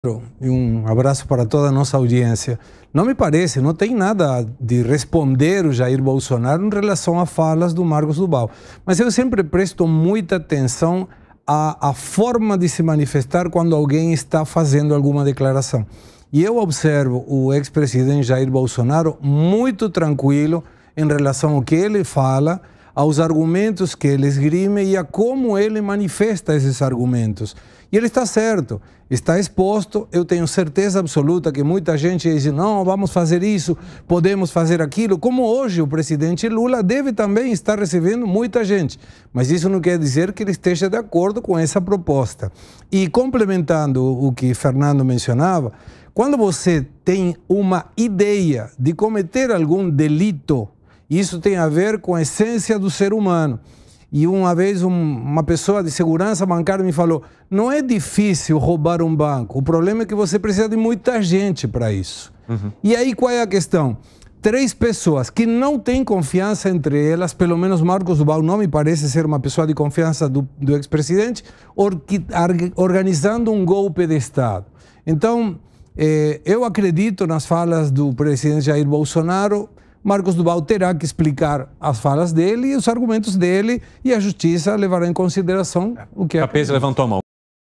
Segré: “Eu acredito nas falas de Jair Bolsonaro”
No 3 em 1 desta quarta-feira, Gustavo Segré destacou que acredita nas falas de Jair Bolsonaro em relação ao seu envolvimento com o senador Marcos do Val. O ex-presidente prestou depoimento à PF sobre uma suposta trama de golpe de Estado.

   / jovempan3em1